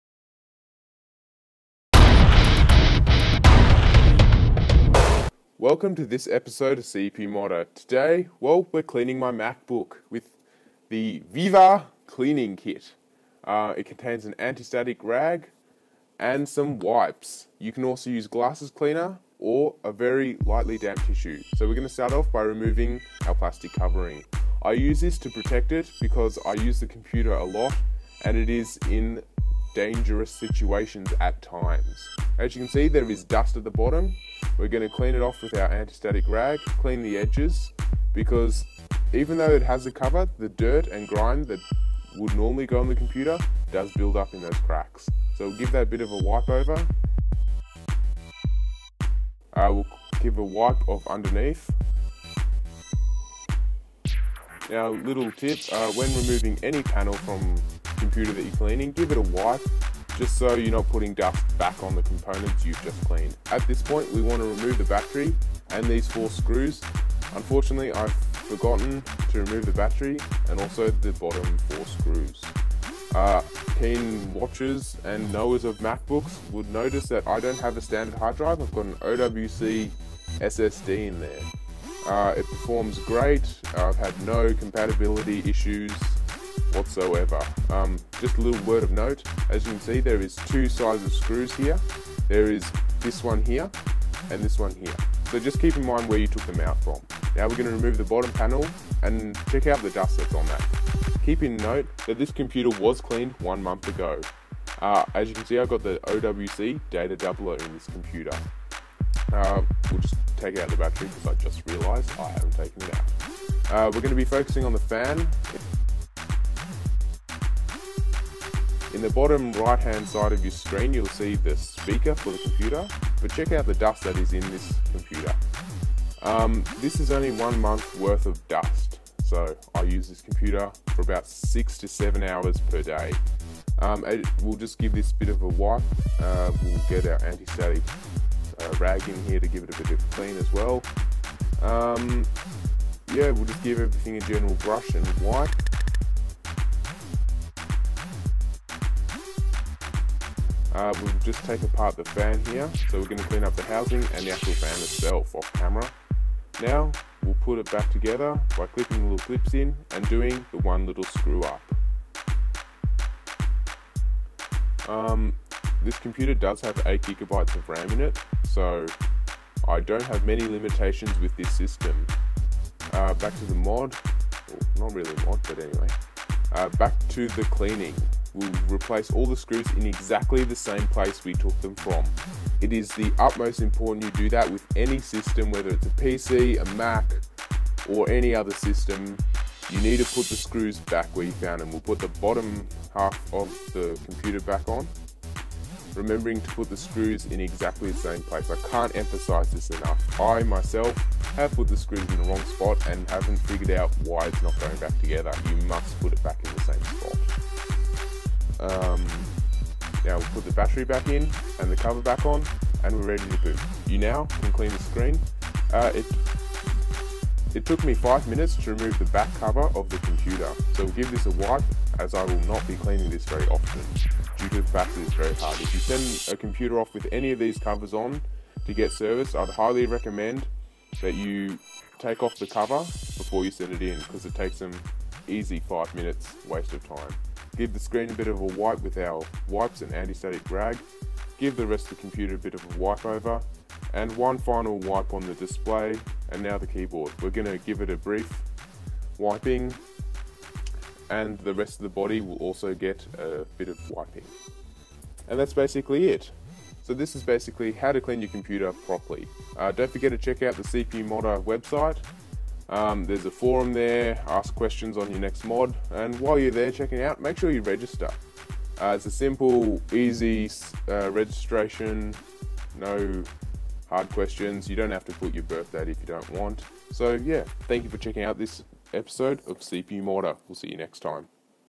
How To Clean A MacBook: CPU modder
!!!WARNING!!!: CPU MODDER IS NOT RESPONSIBLE FOR ANY DAMAGE TO YOUR MACHINE, THIS IS AN ENTERTAINMENT VIDEO only.!!!

today we work on our macbook.

if you want to know the specs of the laptop they are:
mac book 13" aluminium
120gb SSD + 500 gb HDD
2.4 GHz core 2 duo
modded 8 gb ram
NVIDIA GeForce 9400M 256 MB
custom made long life battery
custom heat sink and fan assembly